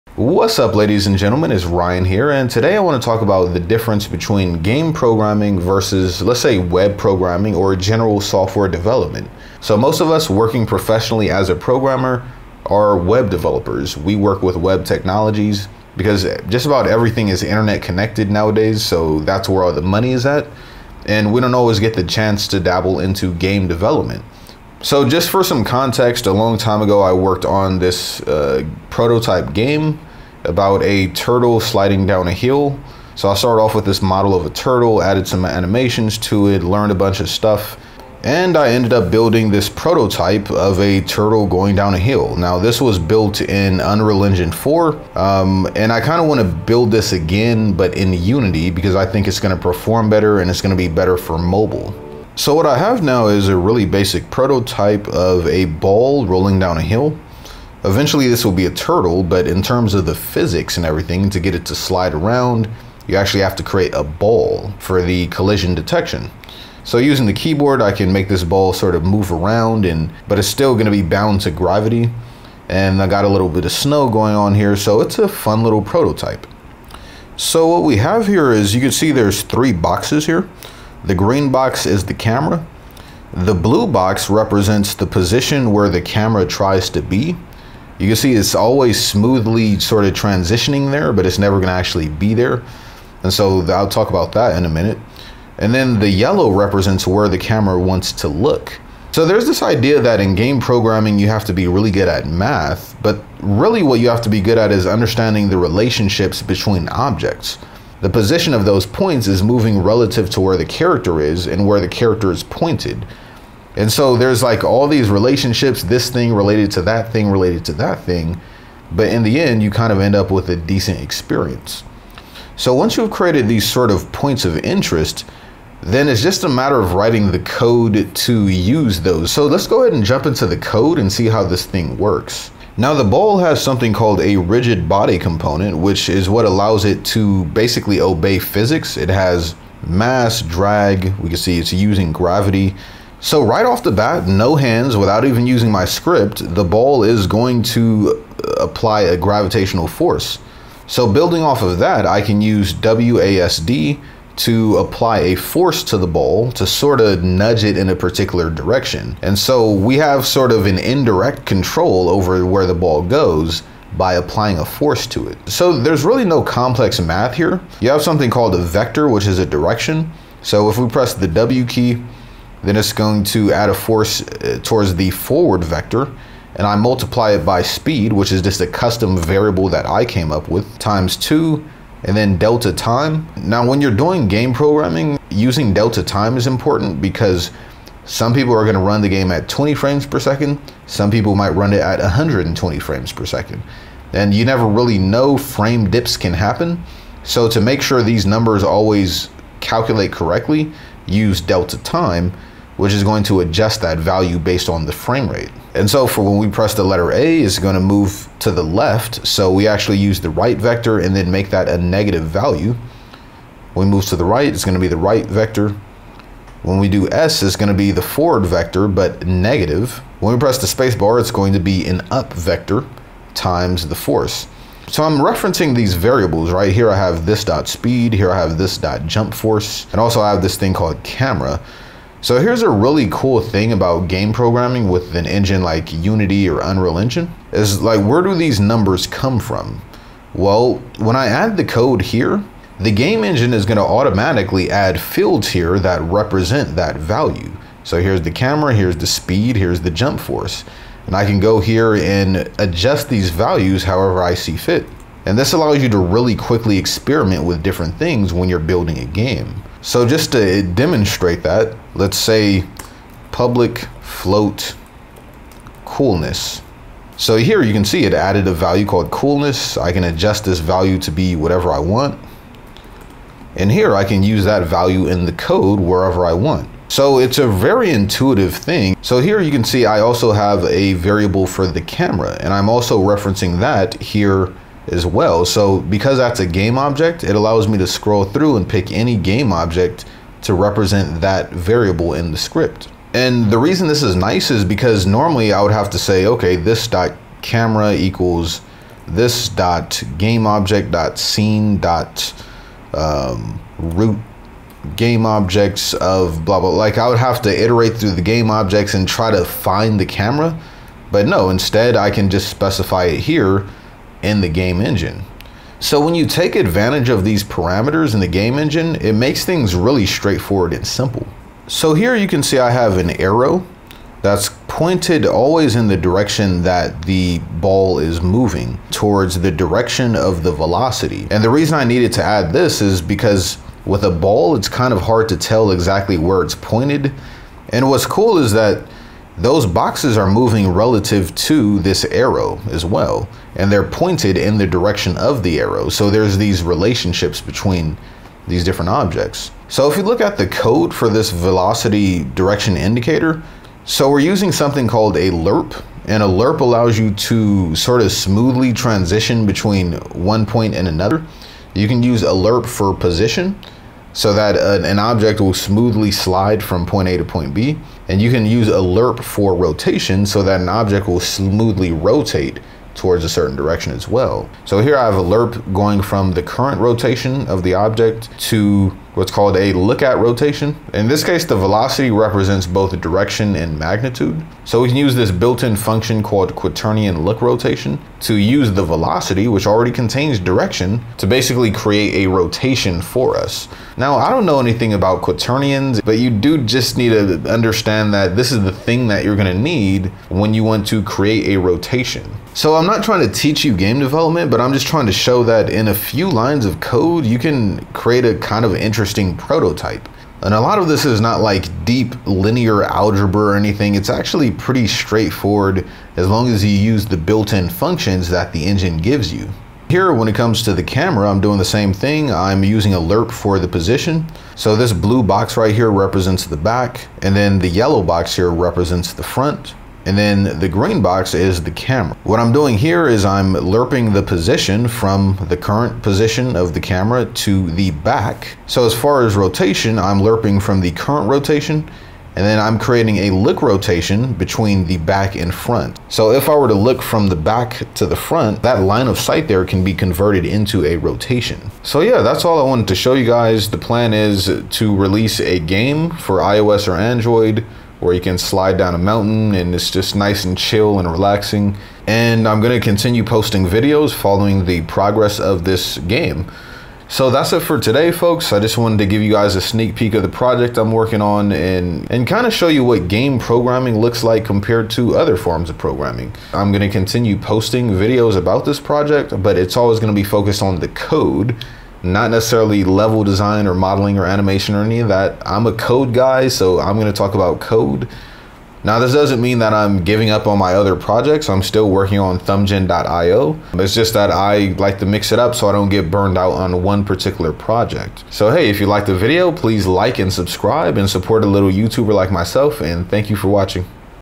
Game Programming From a Web Developer Viewpoint – Unity C# Game Project Devlog PT.1
As I start a new Unity C# game project, I want to talk about how game programming is different than other kinds of programming.

This is part 1 in a series where I cover the programming side of my Unity mobile game project.